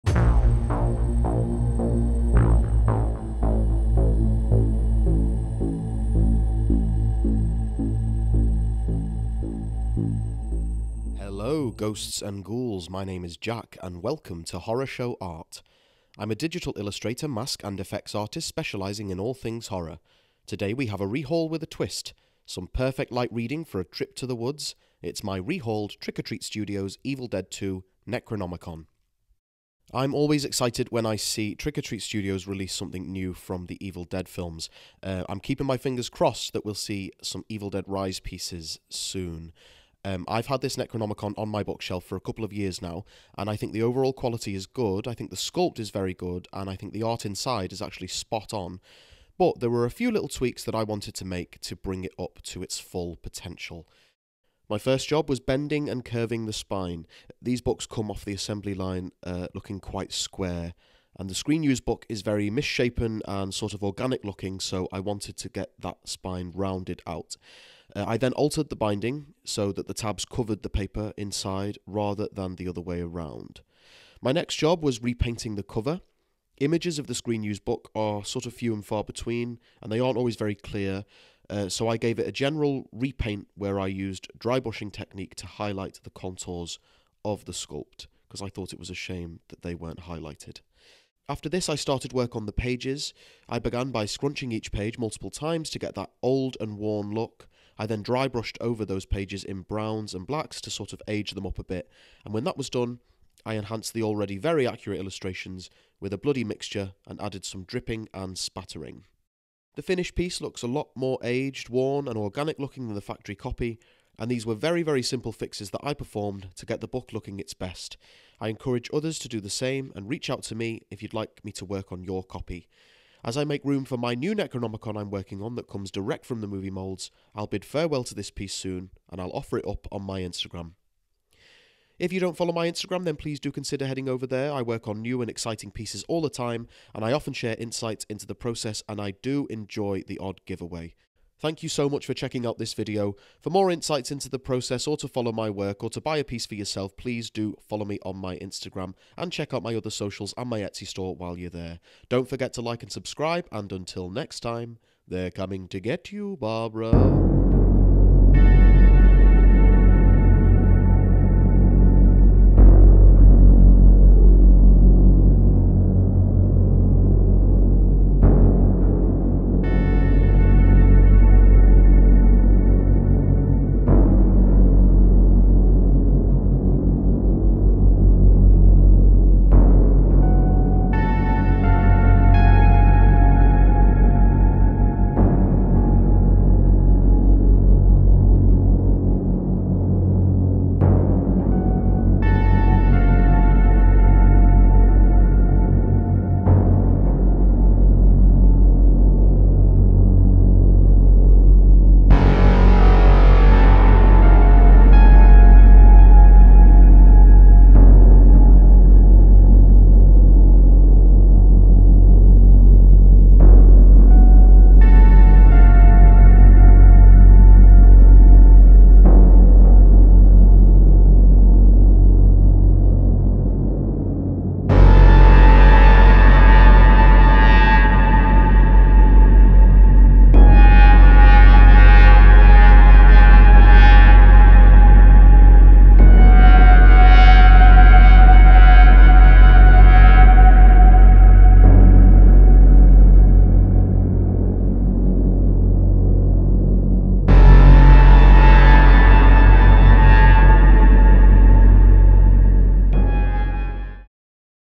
Rehauling my Trick or Treat Studios Necronomicon | Horrorshow Art | Evil Dead 2
Showcasing my rehauled Trick or Treat Studios Necronomicon. To find out how you can get one just like this, to get an inside look at the process or to simply see more of my work you can follow my Instagram here: